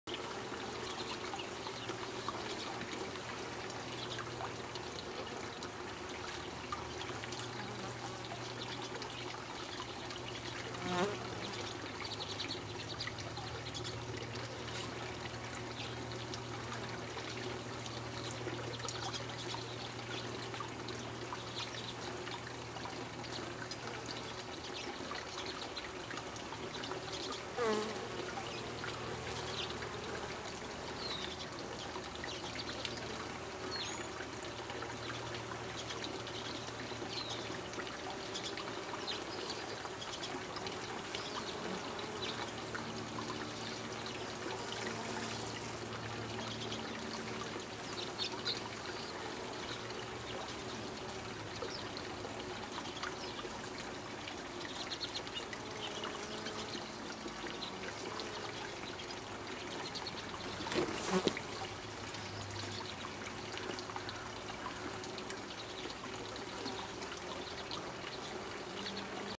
Honeybees removing a dead male drone from their hive
It's early spring. The weather is warming up. Dead and dying drones litter the ground around this hive. Making me worry that the hive had been infected. This video was taken on 3/23/13. On the next day this hive split and swarmed. The swarm of bees settled in a nearby shrub.